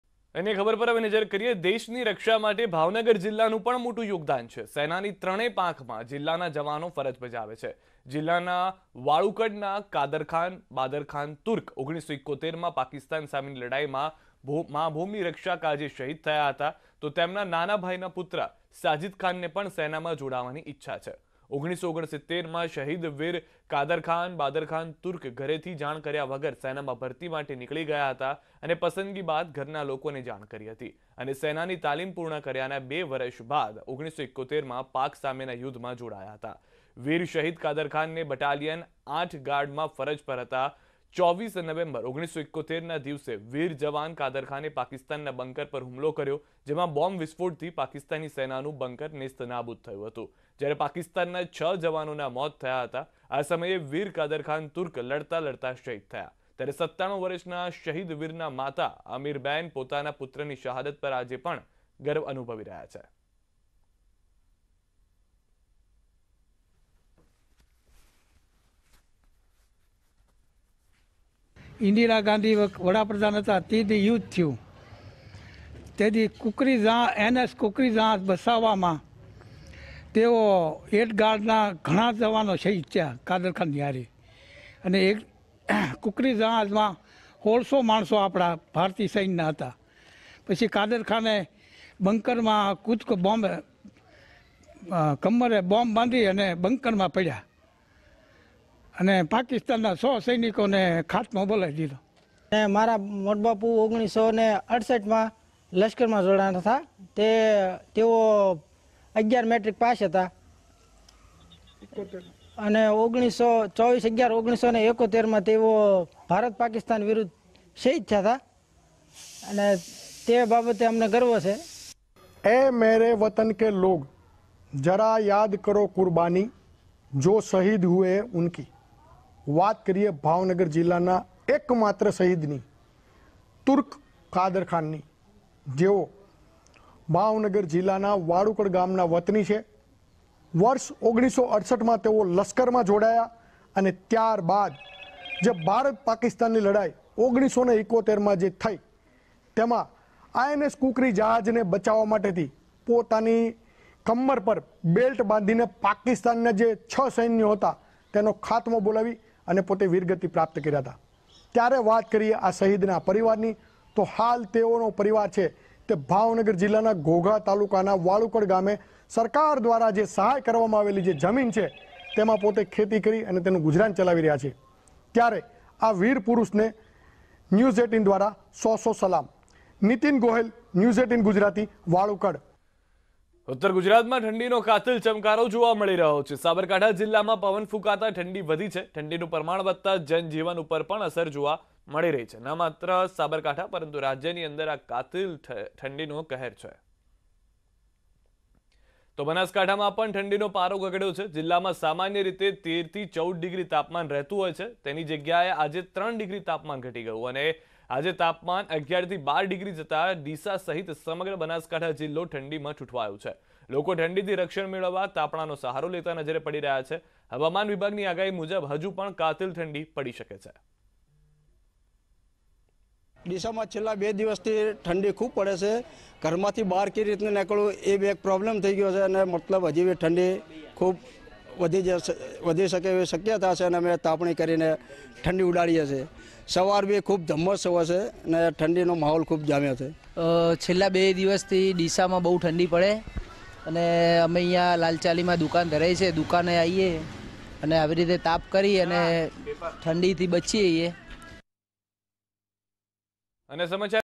Bhavnagar | દેશની રક્ષા માટે Bhavnagar નું મોટું યોગદાન | કેવી રીતે? | News18 Gujarati
દેશની રક્ષા માટે Bhavnagar નું મોટું યોગદાન. દેશની ત્રણેય સેનામાં Bhavnagar ના જવાનોએ યોગદાન આપ્યું છે. શહીદ વીર કાદરખાન, બાદરખાન તુર્ક કે જેમણે 1971 ના Pakistan સામેના યુદ્ધમાં શહાદત વ્હોરી હતી. તેમના વિષે જાણો વિગતે.